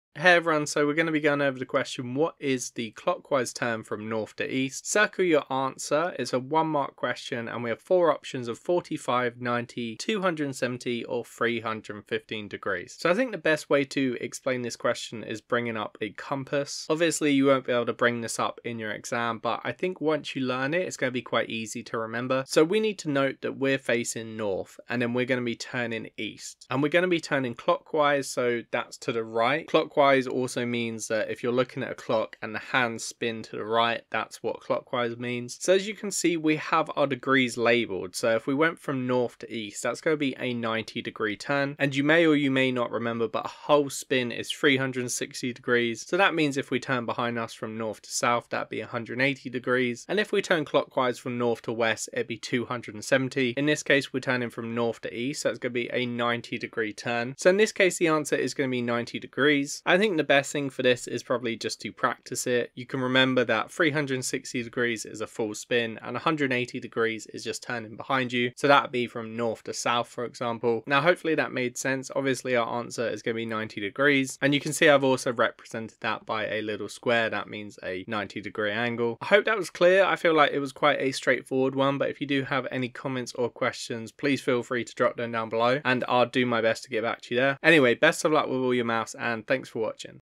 Q1) What Is The Clockwise Turn From North To East? (Degrees)
In this video we look at Q1 from past paper GCSE Mathematics AQA Foundation Tier Paper 3 Calculator November 2022 Q1 (Copy Of Paper):